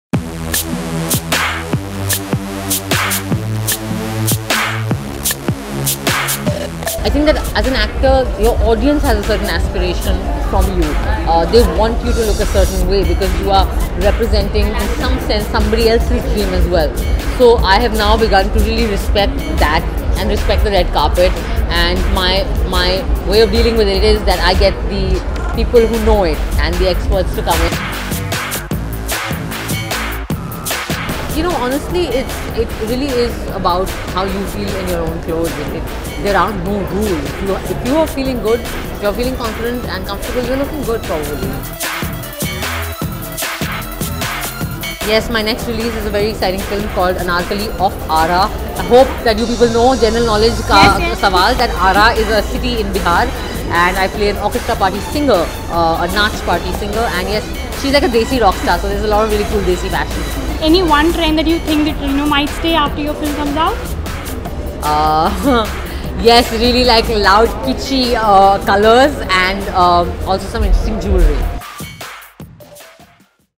Lakme Fashion Week 2017: Swara Talks About Fashion And Her Upcoming Film
Excited about her next release Anarkali of Aarah, Swara Bhaskar shares with News18 her idea of fashion and the fashion trends to look forward to in her upcoming movie.